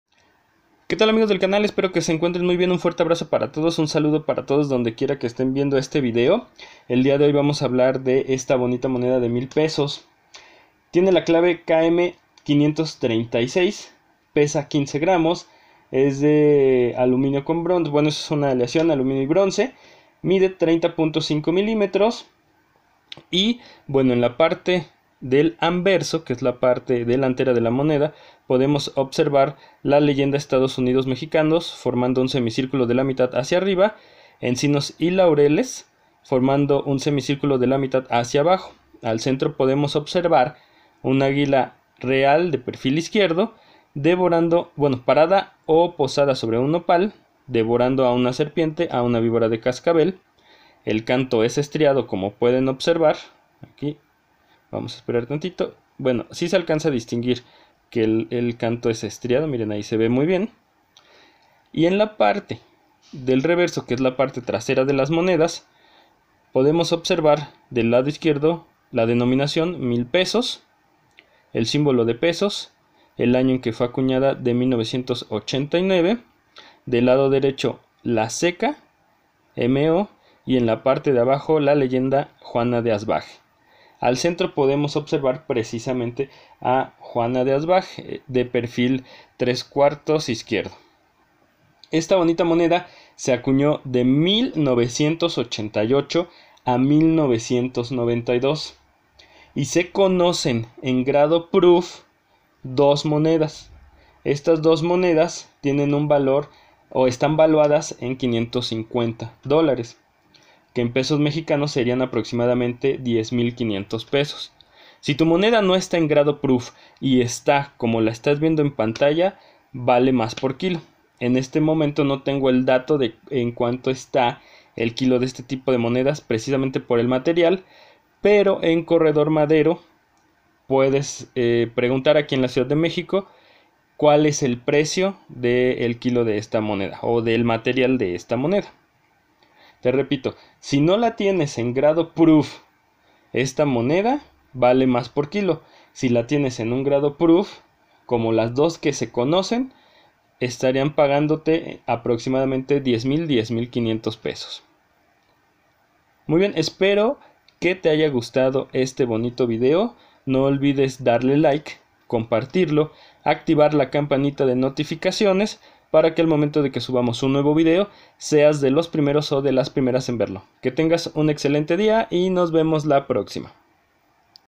Moneda $1000 Pesos Juana de Asbaje / monedas mexicanas / monedas de mexico / sor juana / proof
KM # 536 1000 PESOS Años de Acuñación: 1988 a 1992
15.0000 g., Aluminio-Bronce, 30.5 mm. Tema:
Juana de Asbaje Obv: Armas nacionales, águila izquierda Rev:
Busto 1/4 a la izquierda con valor diagonal en el borde izquierdo Canto: estriado
Ceca: Ciudad de México